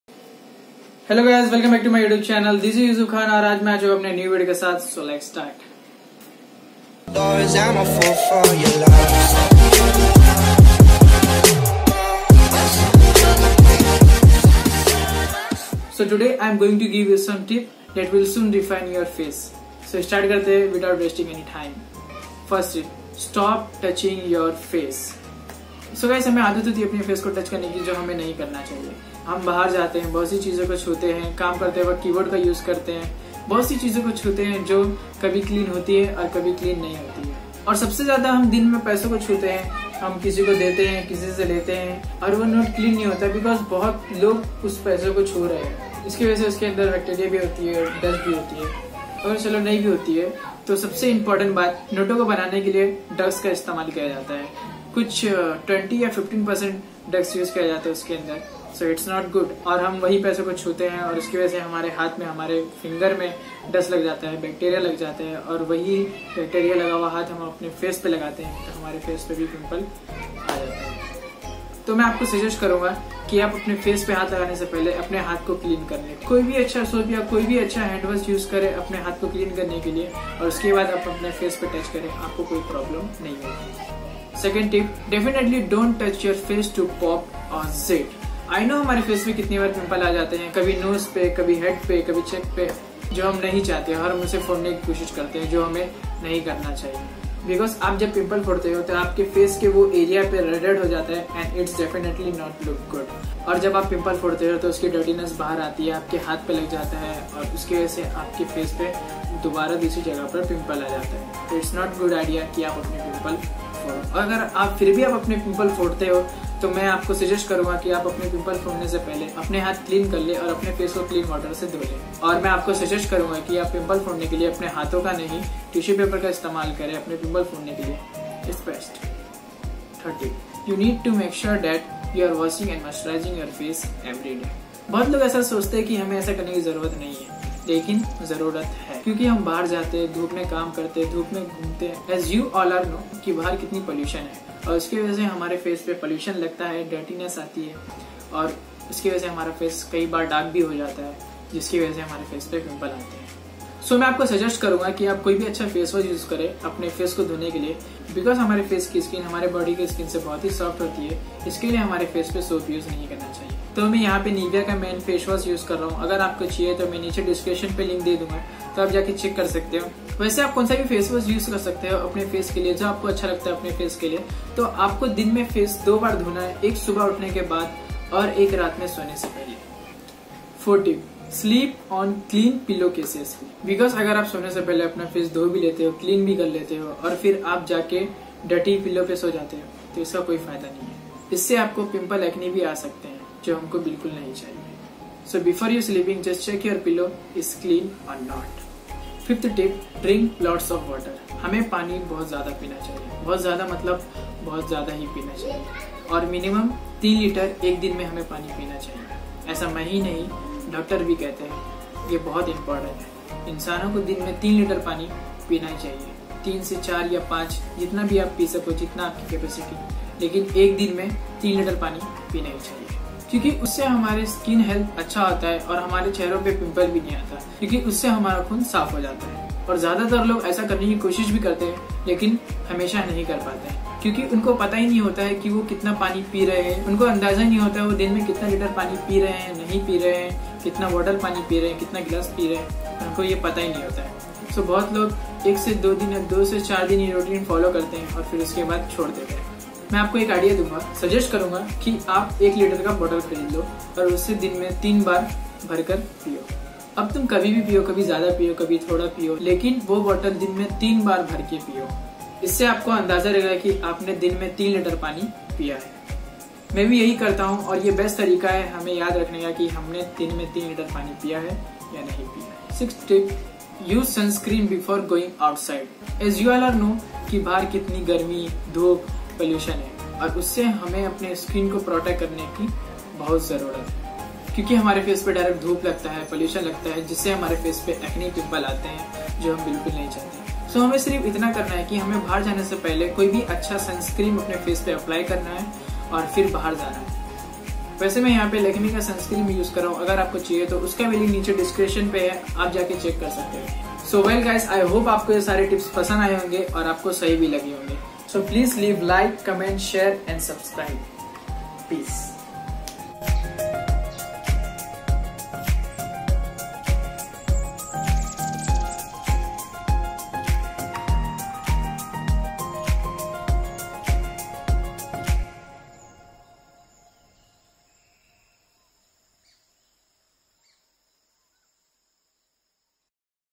HOW TO GET CLEAR SKIN FAST | Men's Skincare Routine | YUSUF KHAN
➤Hey Wassup :)

➤Here are some tips and routine for getting clear skin naturally.

Lakme Sunscreen